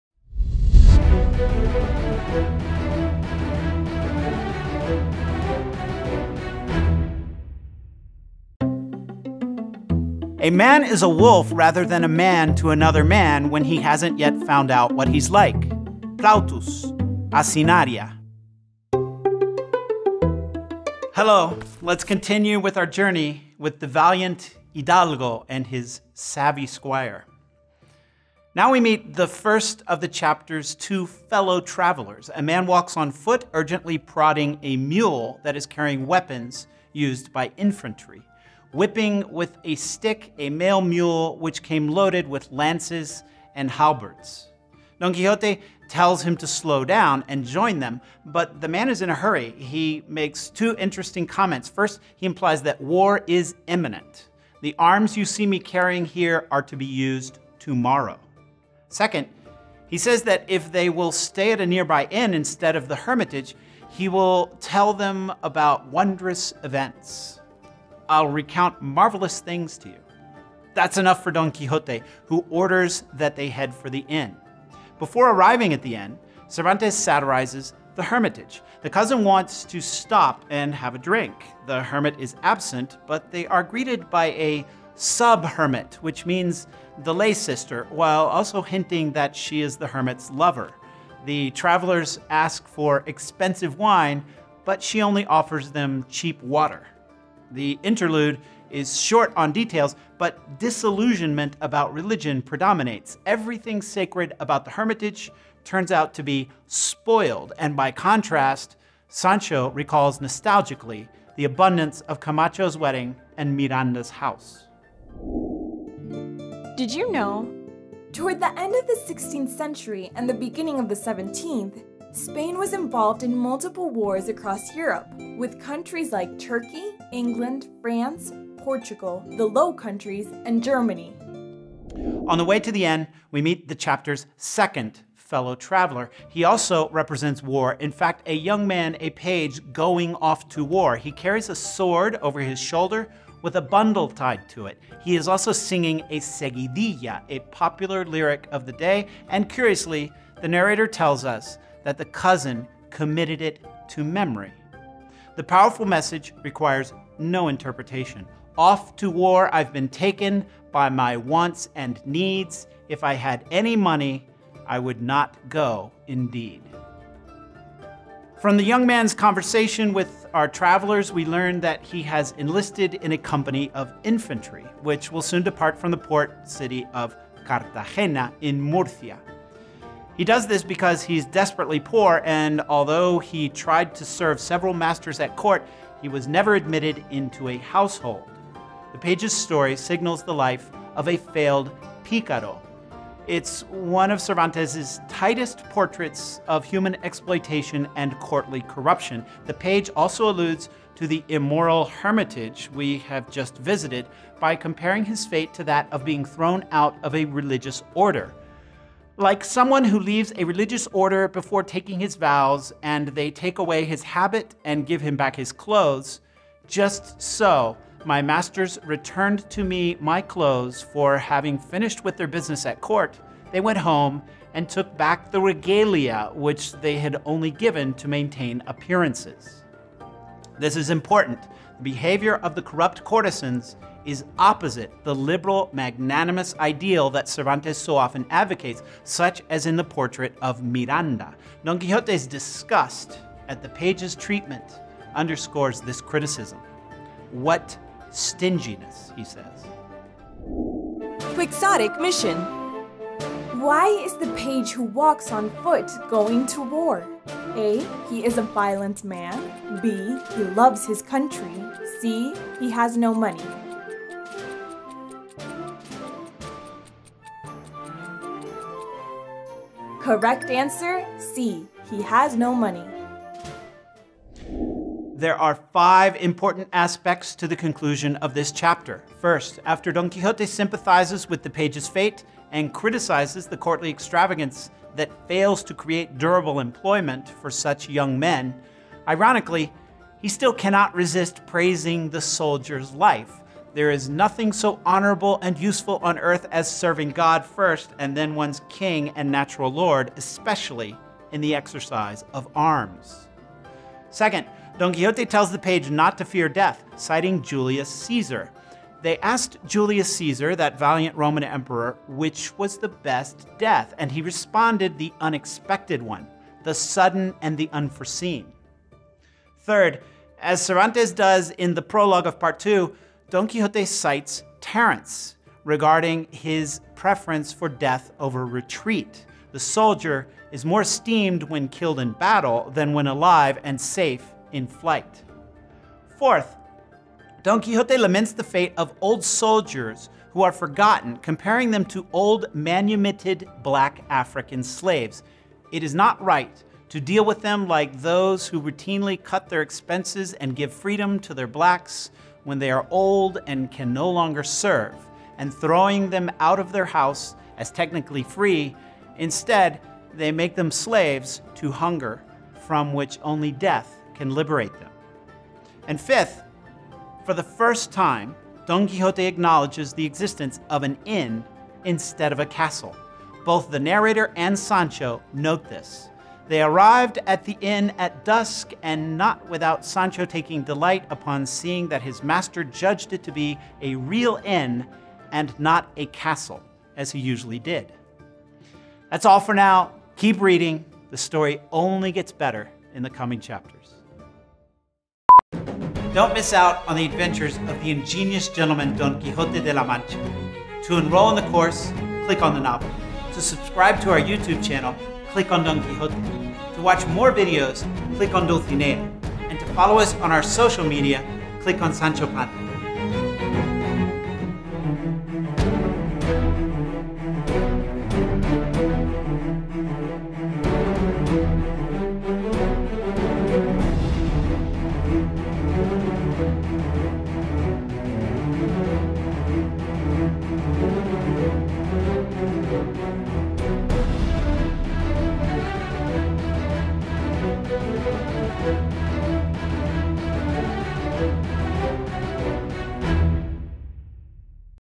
Lesson 2: Don Quijote arrives to the hermitage and then at the inn
MOOC “Discover Don Quijote de la Mancha”

Credits
New Media | UFM 2015